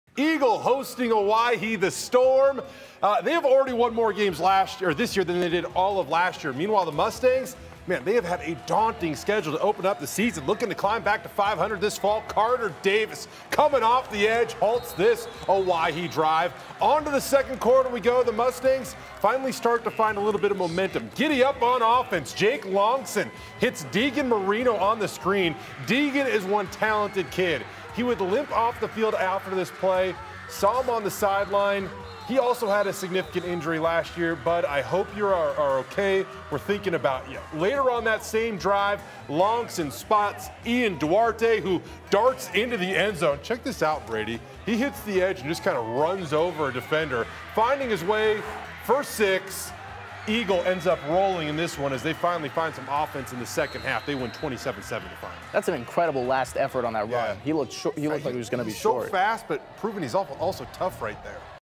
Highlights: Eagle tops Owyhee in KTVB's Game of the Week
The Eagle Mustangs improved to 2-2 Friday night, defeating the Owyhee Storm (3-2) in KTVB's Game of the Week 27-7.